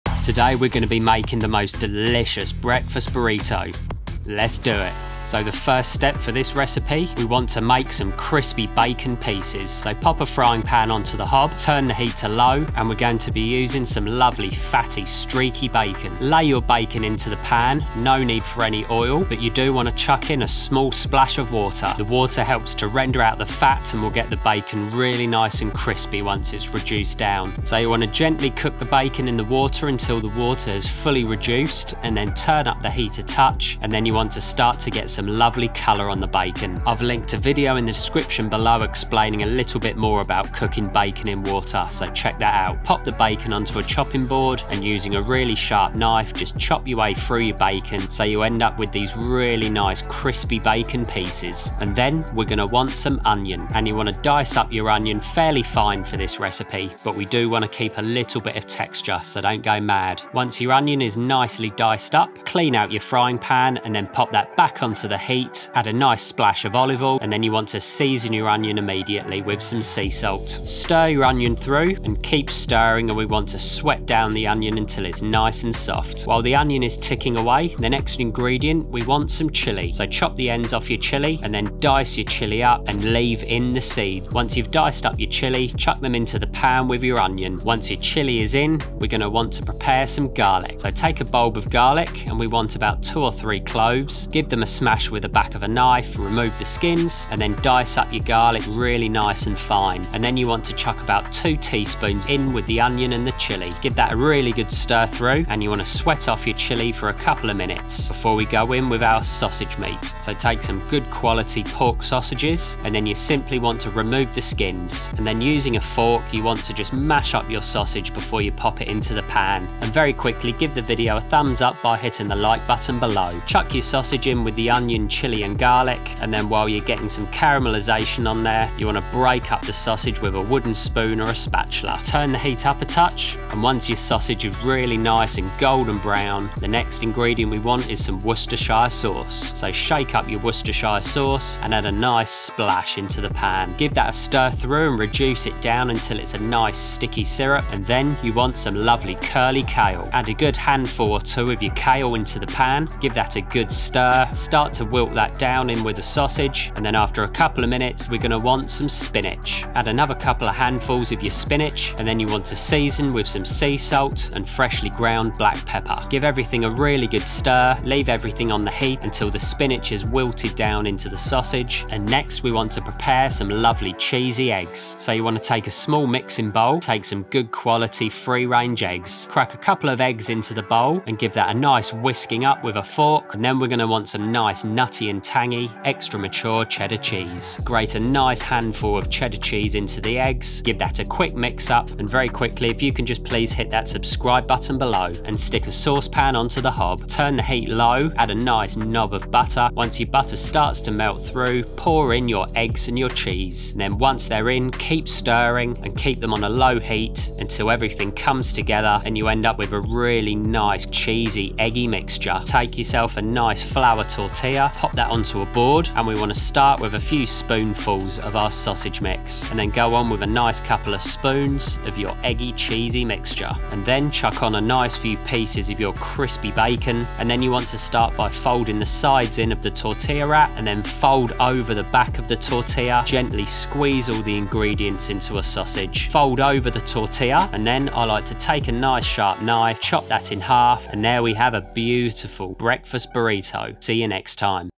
This Burrito will change your Breakfasts FOREVER
How to make a delicious Breakfast Burrito at home. Learn my tasty Burrito recipe for Breakfast. The recipe uses indulgent Cheesy Eggs, spicy Sausage and crispy Bacon. This video will teach you how to make the most amazing Breakfast Burrito wrap. Learn how to cook a delicious spicy Sausage filling to go with some amazing crispy Bacon pieces and some gooey and Cheesy scrambled Eggs, then How to roll a Burrito the right way. This recipe is so easy, so delicious and will change the way you Breakfast forever.

My recipe in this video is very easy, quick and fool proof to prepare and cook. Anyone can make this tasty and delicious dish for a date night, a dinner party, to feed the whole family, or to impress your friends. This recipe is hearty and rustic, and so so tasty.